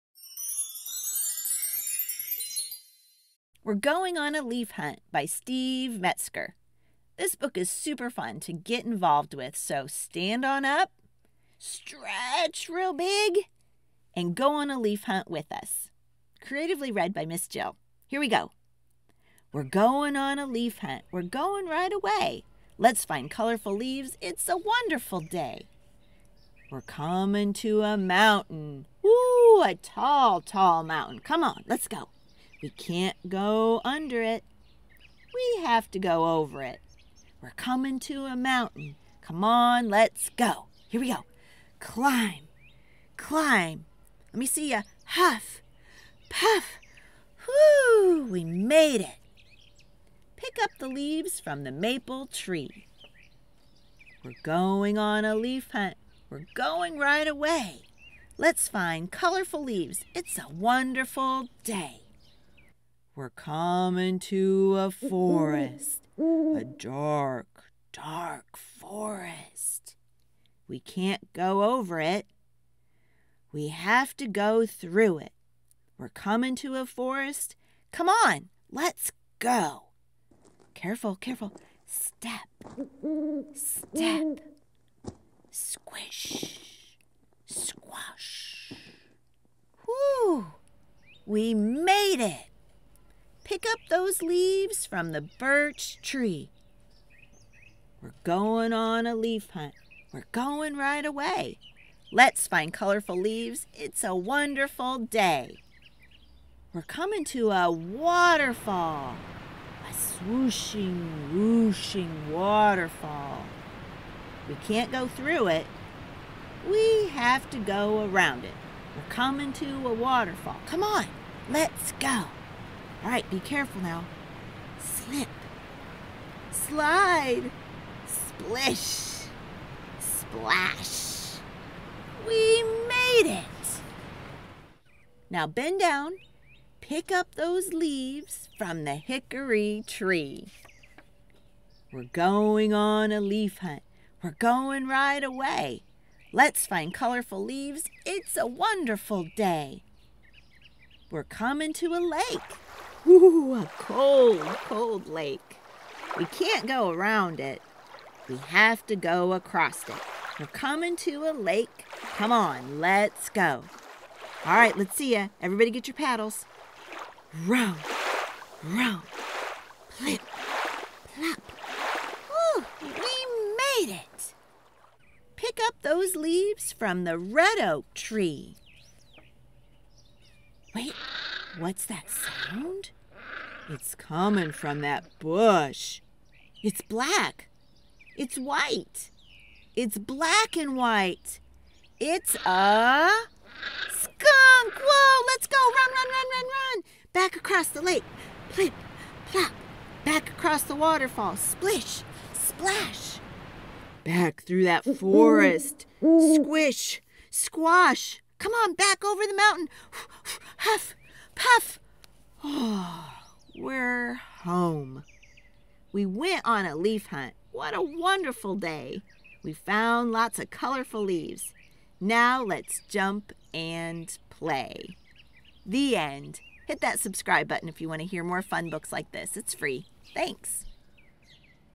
🍁Fall Books |We're Going on a Leaf Hunt (Read Aloud books for children) | Pretend Play Miss Jill
🍄Can you feel the cool autumn breeze? The musty smell of freshly fallen leaves? It's time for a Leaf Hunt! So get up and follow along with the actions! Join us for the fun in this rhythmic dramatic read of Going on a Leaf Hunt 🍁by Steve Metzger read by Miss Jill.